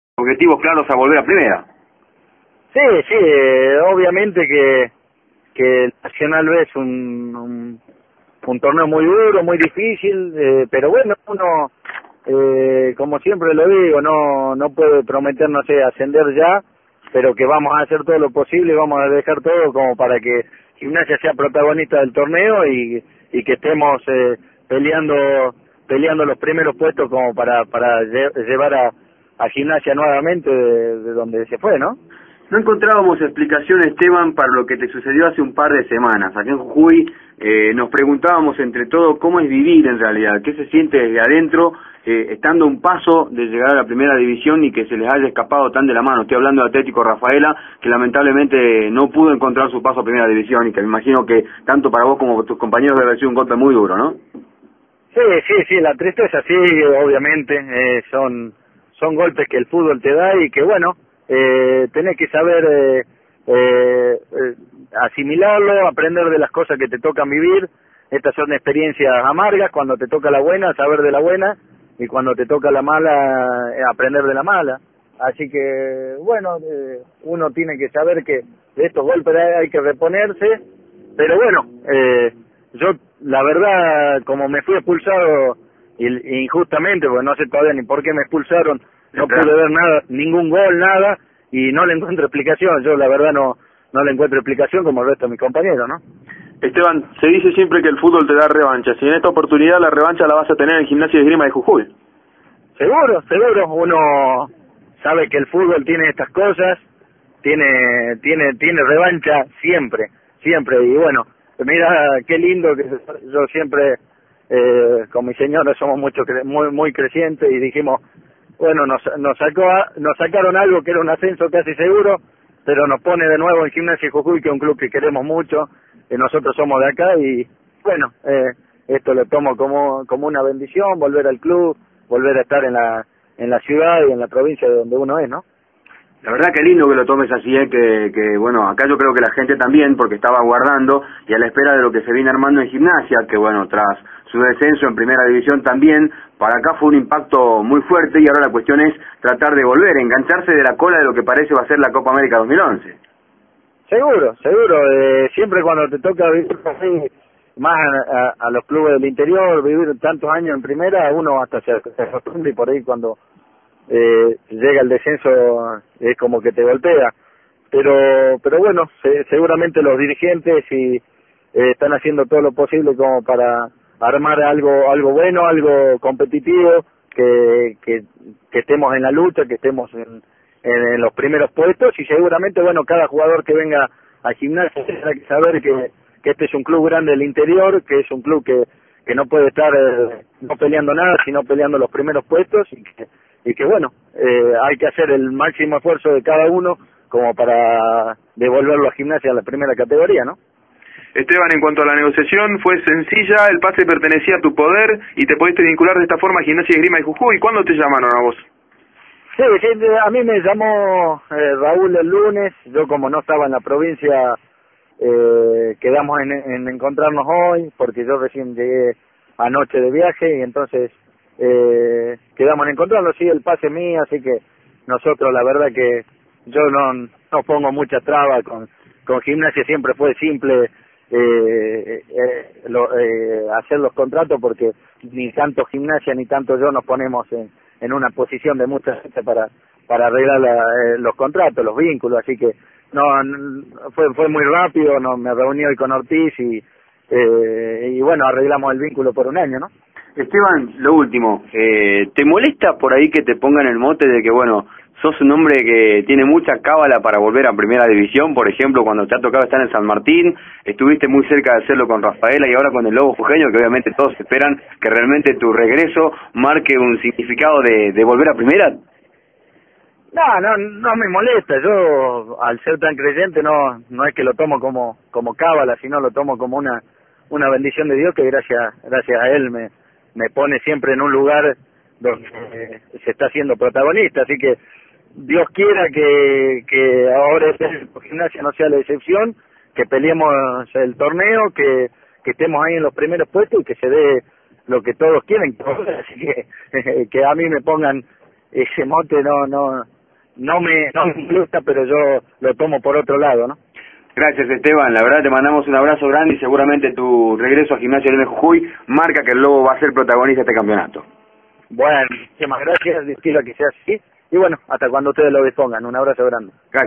ESTEBAN GIL
EN DIALOGO CON RADIO NOA , ESTA CONTENTO POR VOLVER A JUJUY